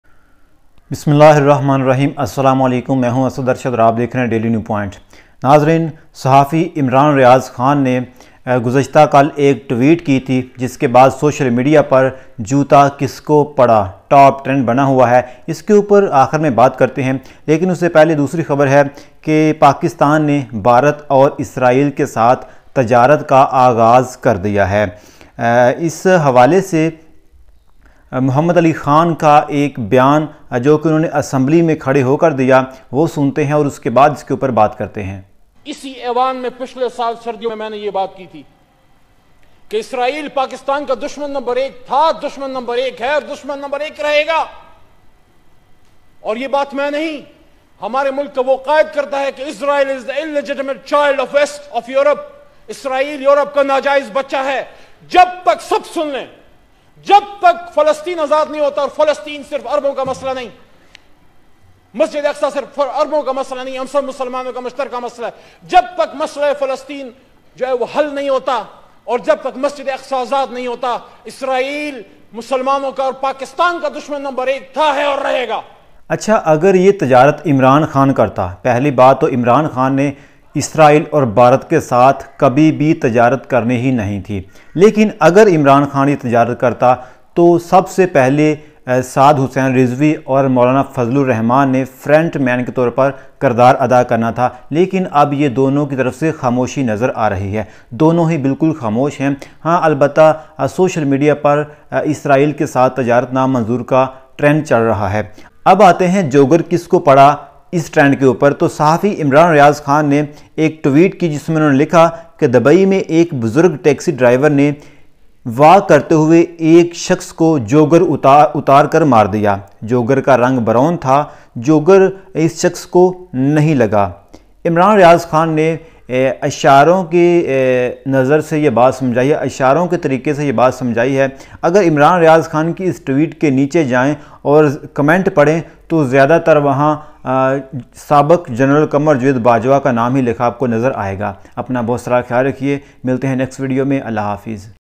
Dubai Main Juta Kis Ko Prha ? Anchor Imran Riaz Khan Reveals Story In Today | Pakistan Trade Israel
Dubai Main Juta Kis Ko Prha ? Anchor Imran Riaz Khan Reveals Story In Today | Pakistan Trade with Israel Update